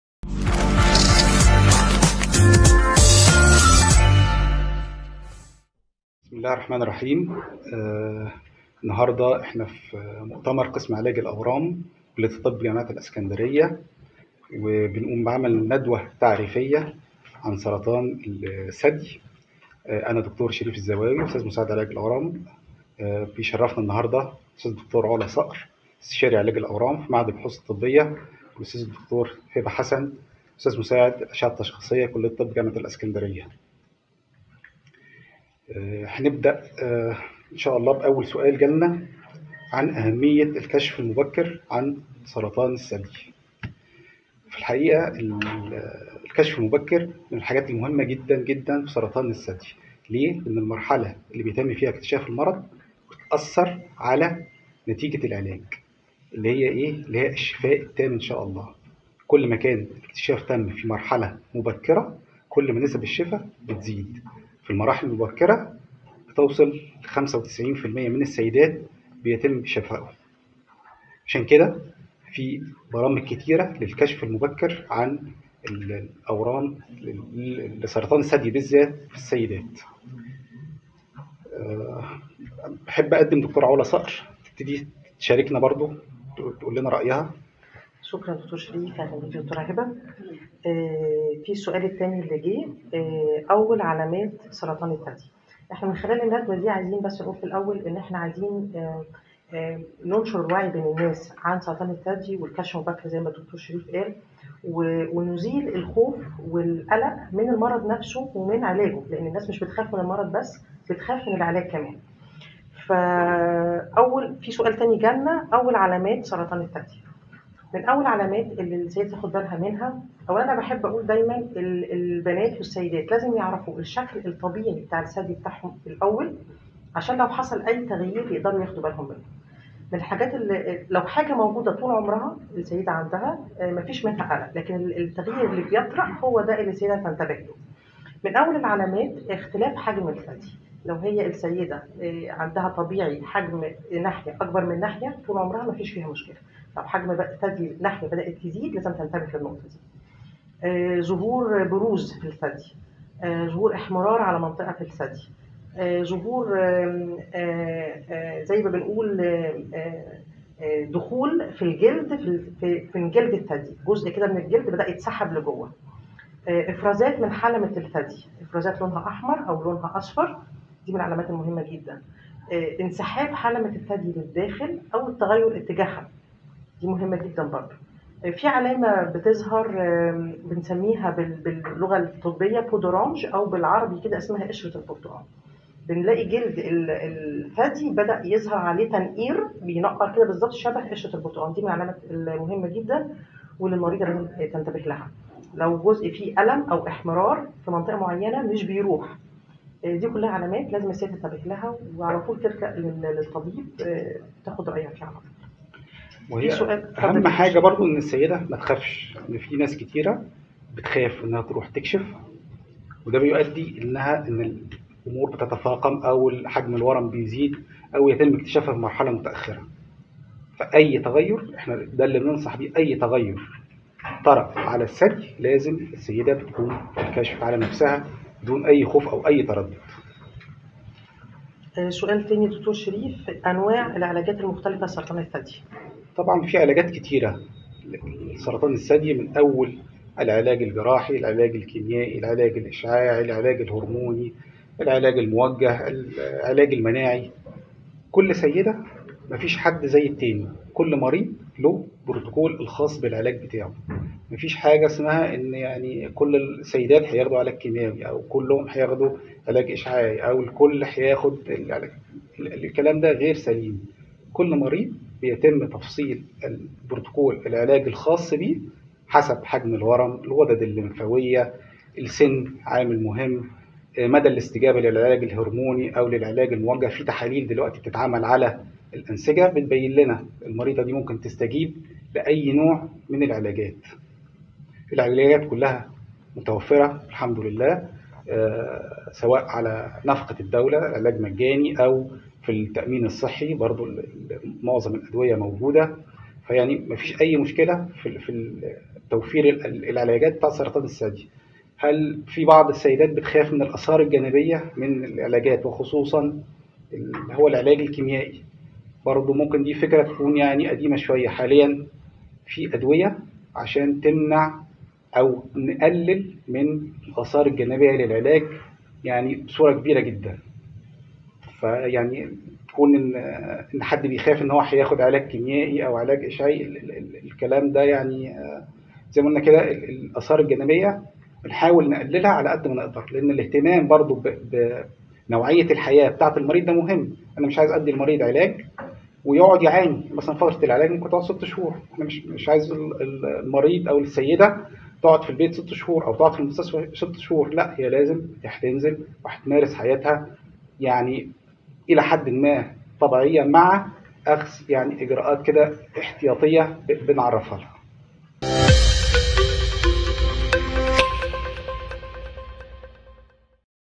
الحلقة الأولى عن الإكتشاف المبكر عن أورام السرطان و أنواع العلاجات - للتوعية لسرطان الثدى
عن الإكتشاف المبكر عن أورام السرطان و أول علامات سرطان الثدى
أنواع علاجات الأورام

الأستاذ الدكتور  شريف الزواوى
أستاذ مساعد علاج الأورام كلية طب - جامعة الإسكندرية

الأستاذ الدكتور علا صقر
إستشارى علاج الأورام و الطب النووى
 معهد البحوث الطبية - جامعة الاسكندرية

الأستاذ الدكتور هبه حسن
أستاذ مساعد الأشعة التشخيصية  كلية طب – جامعة الإسكندرية